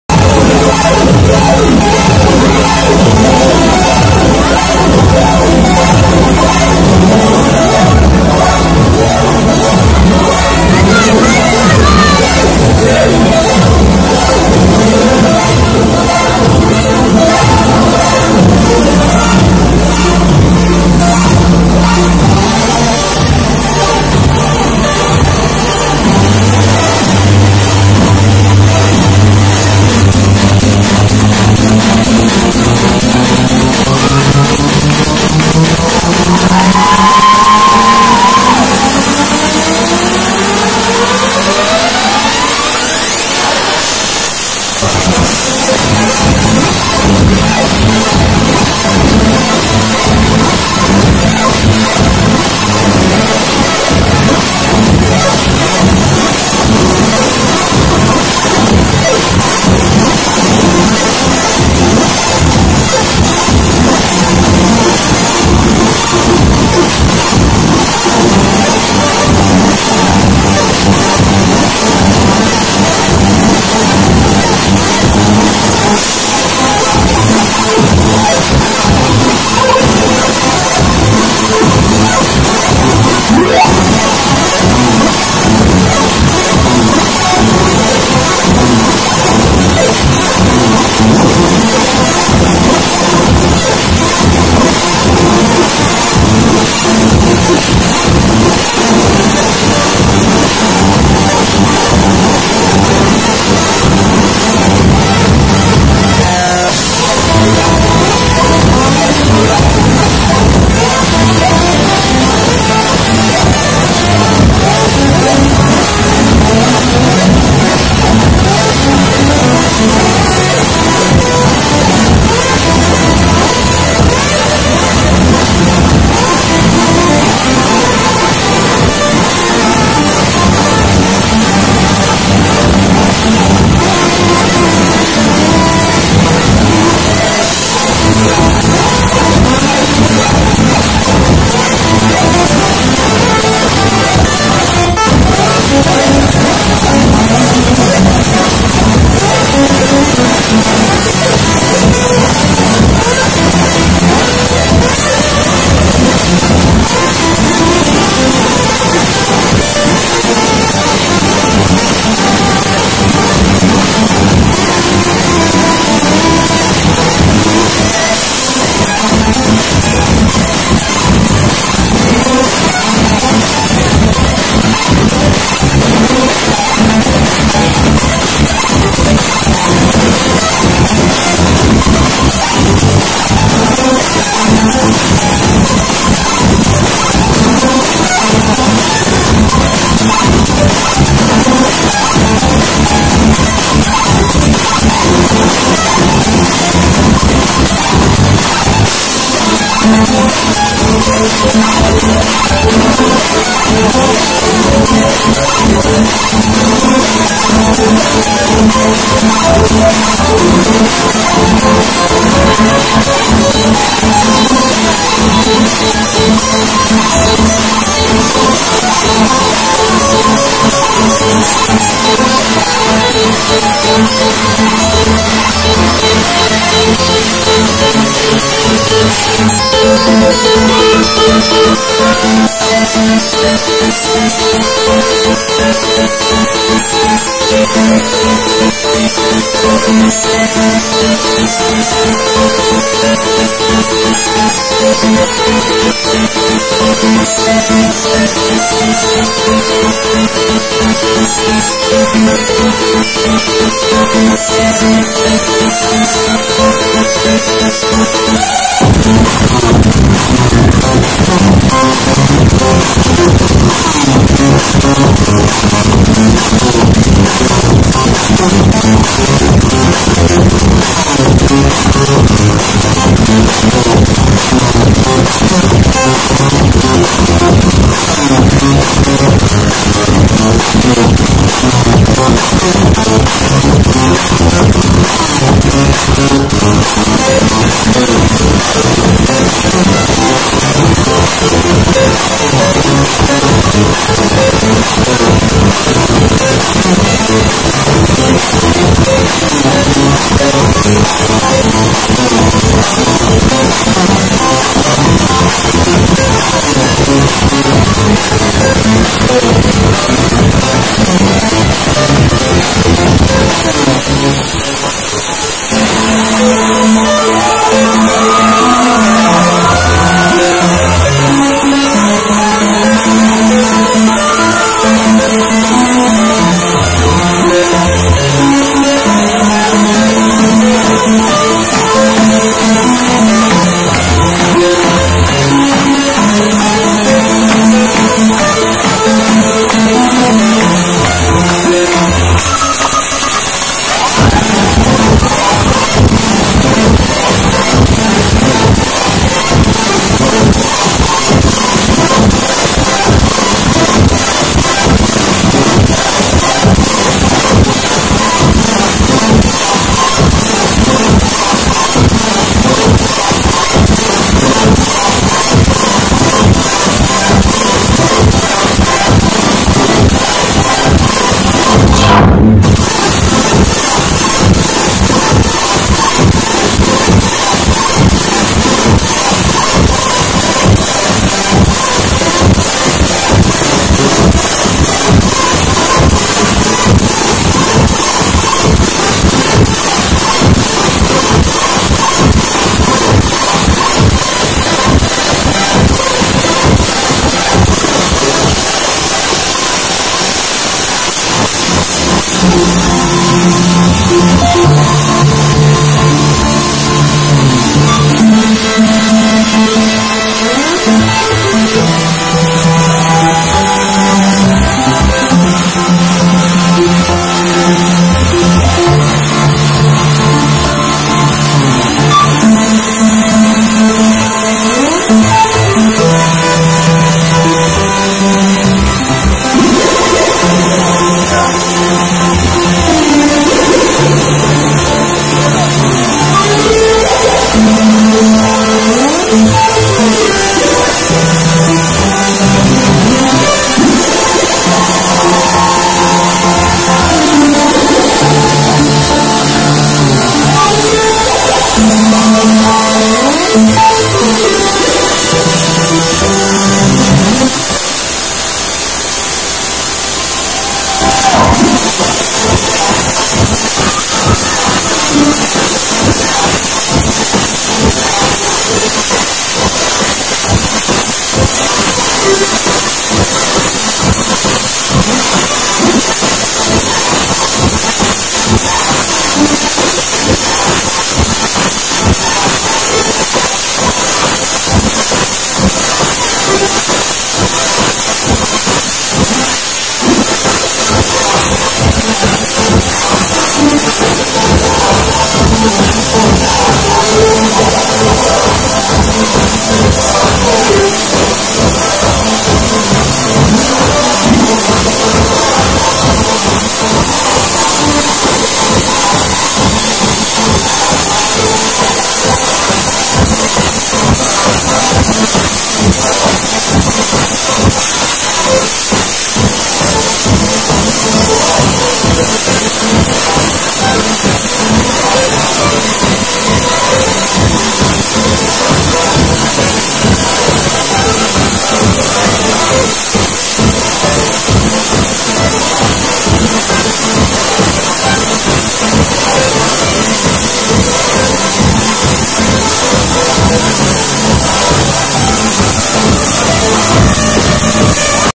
Knife City @ Future Babycastles
From the Future Babycastles "QWOP THE SCREEN" party on 7 September, 2011. It's a little distorted because I was right next to the speaker, and a more than a little shaky because I was rocking out. Fight me.